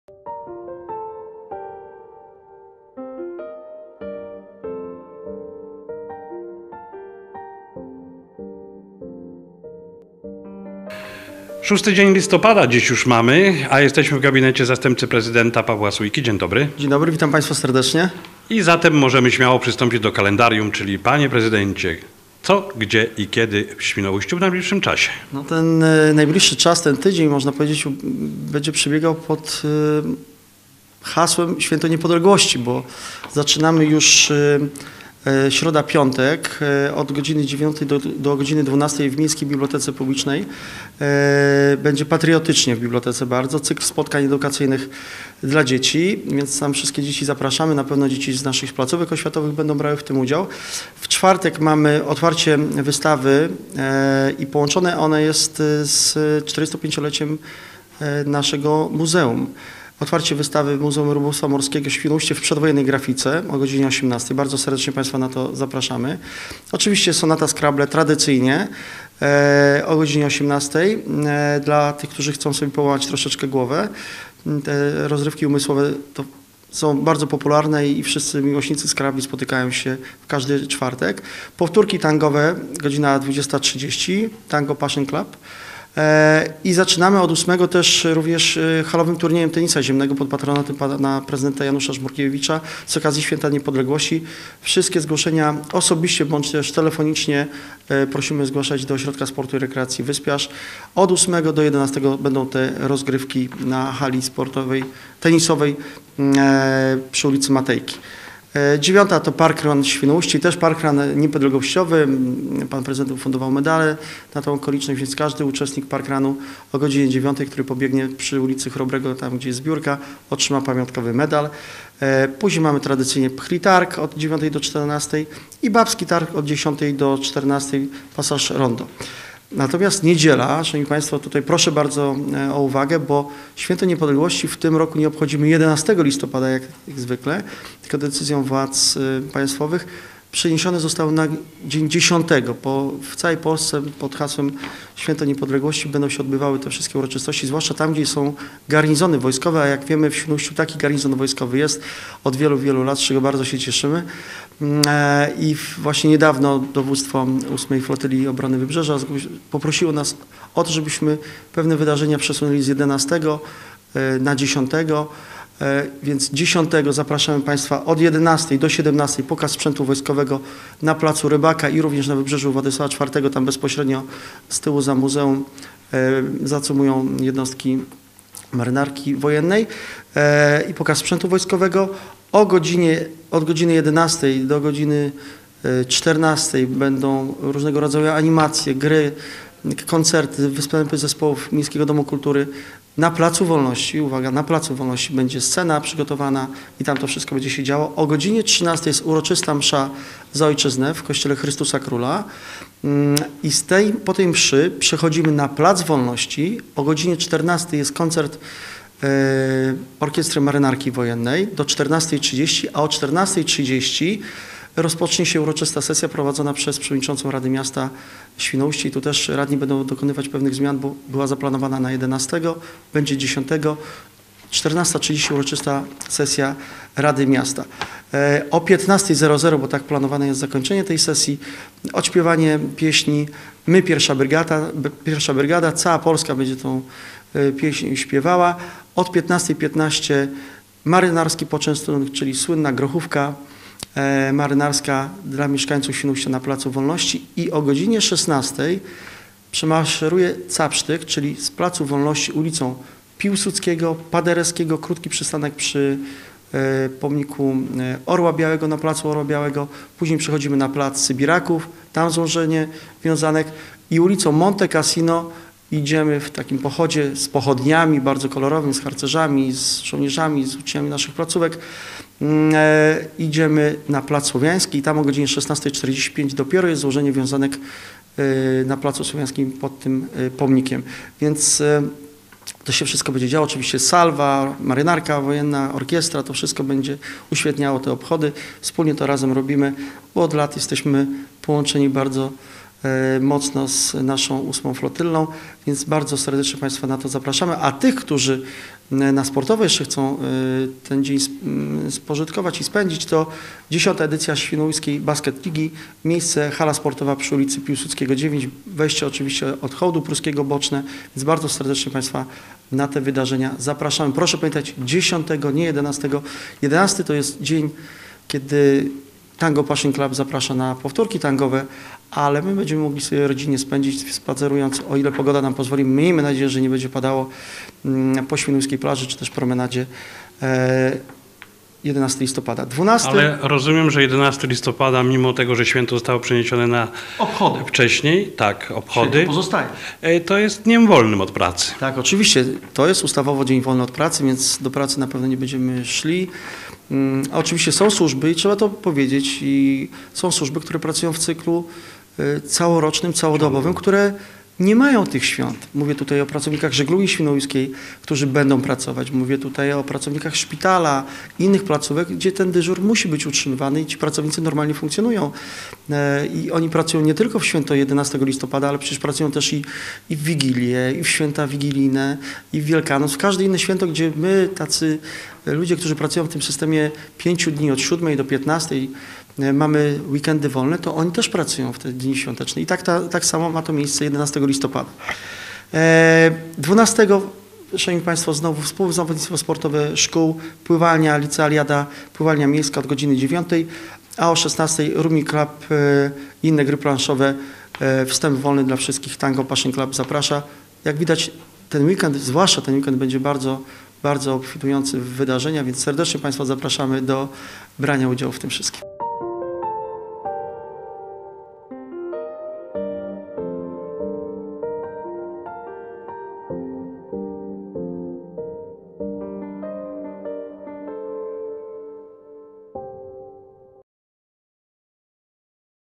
Kalendarium – czyli, Co?, Gdzie? i Kiedy?
Najbliższe uroczystości i przedsięwzięcia odbywające się na terenie miasta Świnoujście zaplanowane zostały pod kątem obchodów 101. rocznicy odzyskania przez Polskę niepodległości.